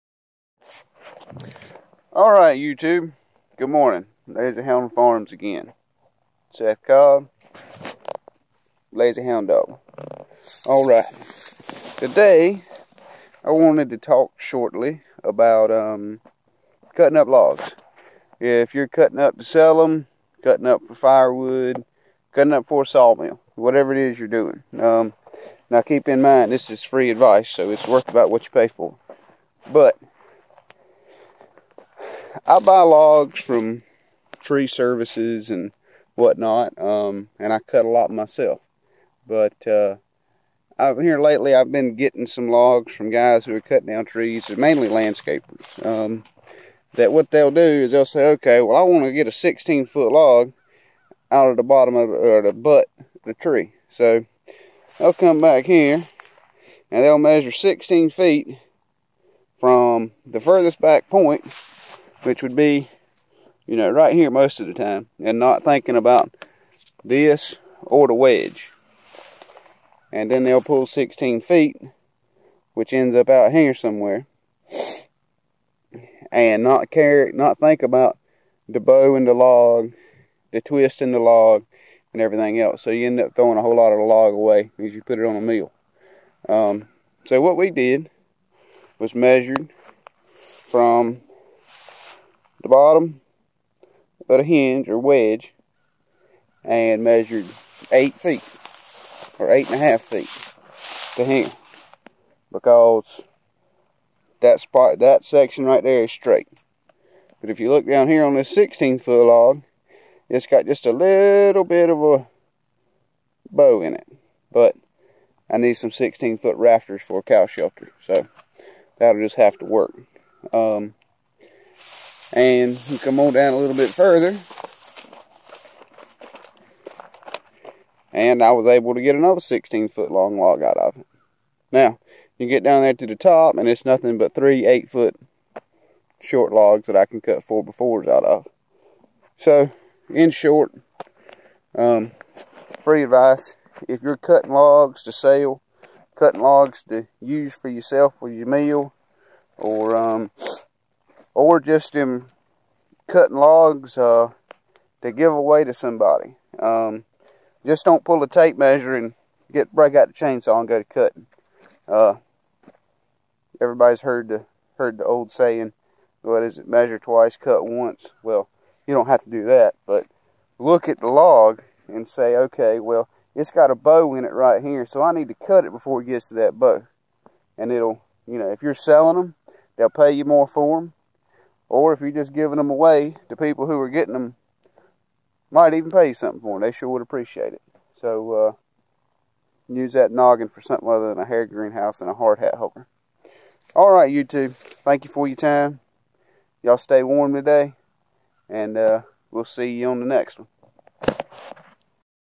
Free advice on how to cut logs
Getting the very most out of your logs important so think before you cut
But just a short video talking about logging bucking and logs the tree in the video is a 30" poplar that I'm using for 4x4s and 2x6 rafters for a cow barn